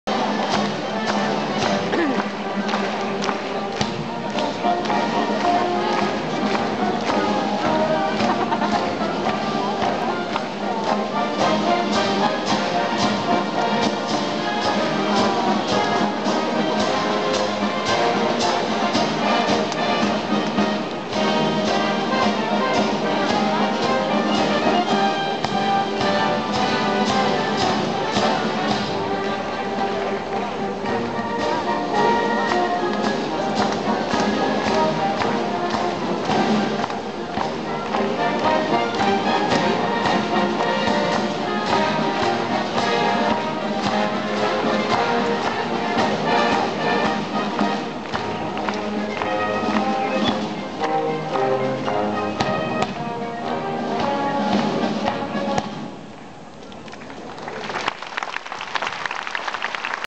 Le bande al "Military Tatoo" a Modena
L'ingresso di tutte le bande che hanno preso parte al "Festival Internazionale delle bande militari di Modena" tenutosi l'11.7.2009